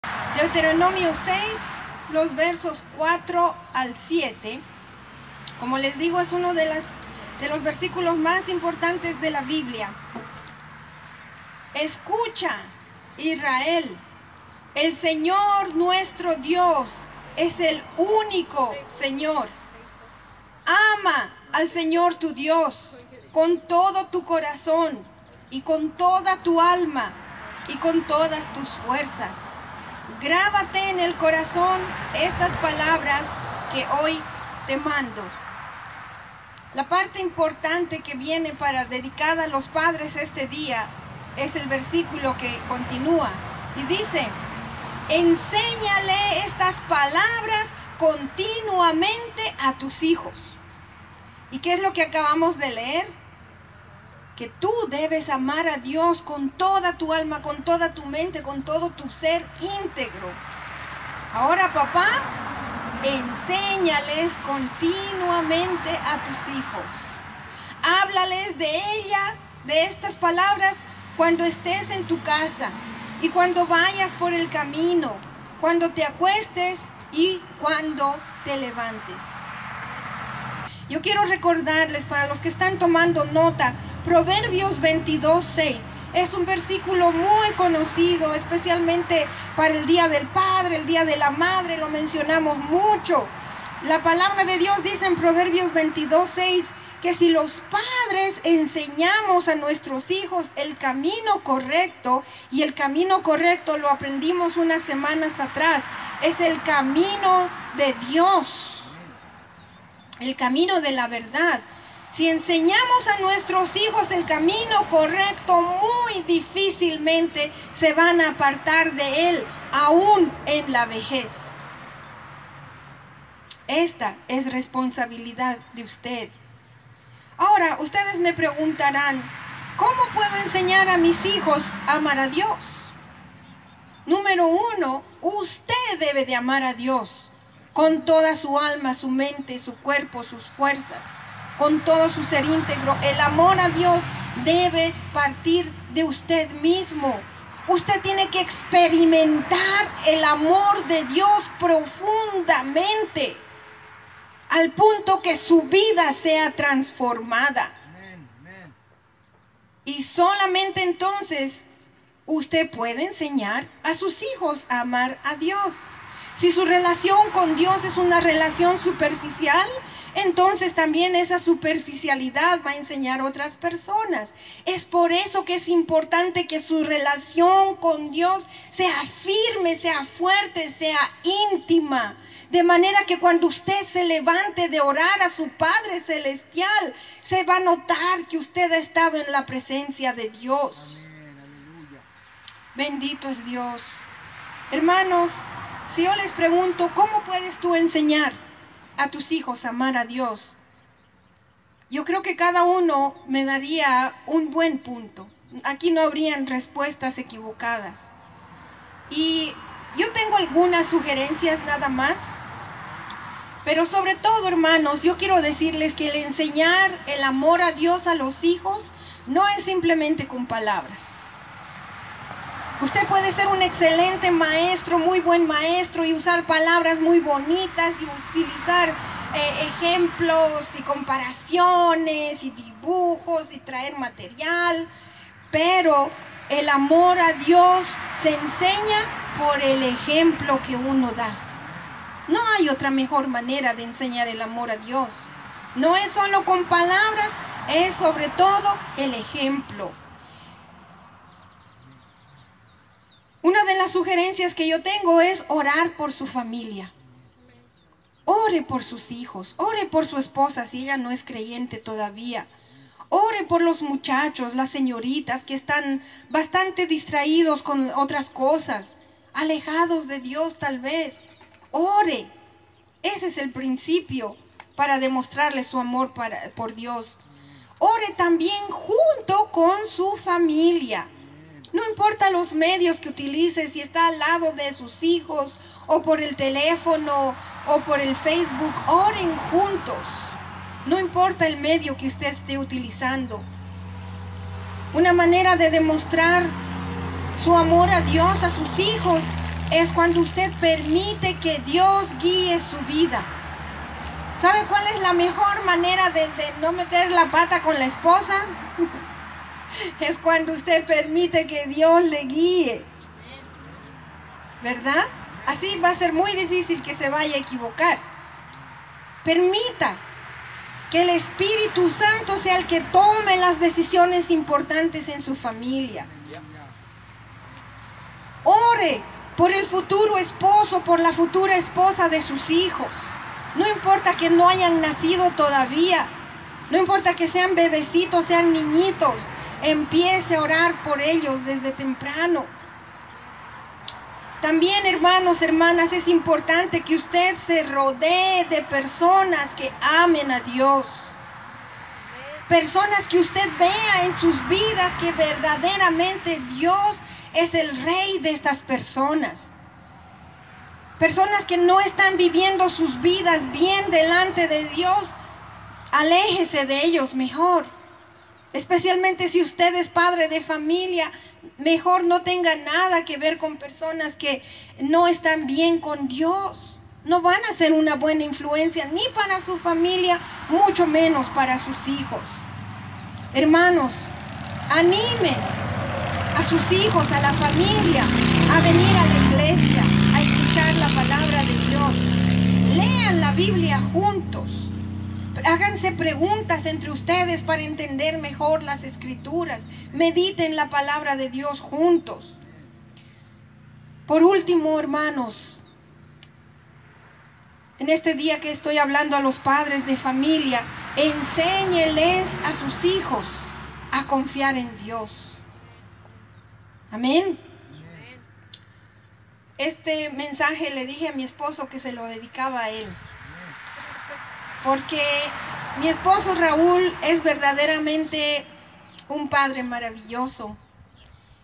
¿Cómo podemos enseñar a nuestros hijos a amar a Dios? Deuteronomio 6:4-7 y Proverbios 22:6
La pastora Luisa Mendoza nospresenta unas pocas ideas para que los padres de familia enseñen a sus hijos a amar a Dios, basada en Deuteronomio 6:4-7 y Proverbios 22:6.
Día del Padre, domingo 21 de junio del 2020.
Iglesia del Nazareno Nuevo Amanecer
16236 N. Nazarene Road
Effingham, Illinois 62401